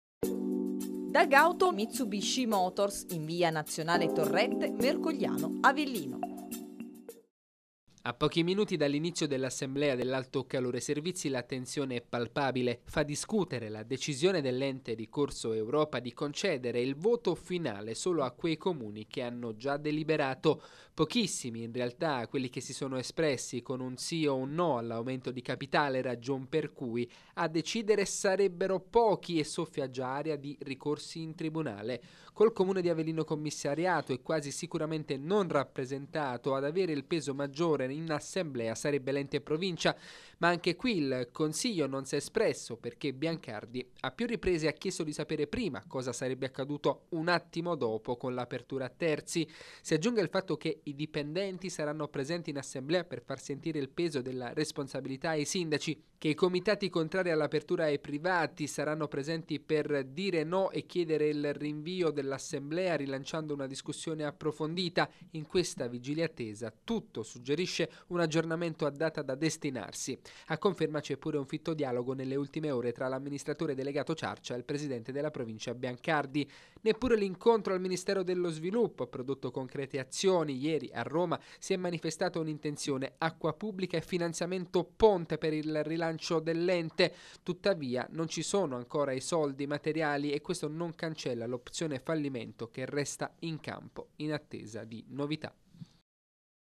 Assemblea Alto Calore, l’ipotesi rinvio prende quota: il futuro resta un’incognita
Dalla vigilia di tensione alla «tregua armata». Potrebbe slittare l’assemblea dei sindaci-soci chiamati a decidere il futuro dell’ente idrico. Fitti colloqui Ciarcia-Biancardi, pressing di sindacati e comitati, minacce di ricorsi, tutto suggerisce un rinvio a data da destinarsi.